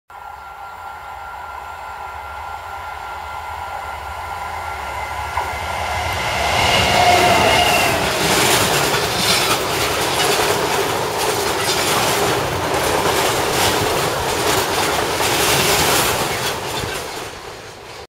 DBS 66104 ON 6E30 COILS AT LEY CROSSING 160713
DBS 66104 on coils from Margam to Hartlepool travelling through Gloucestershire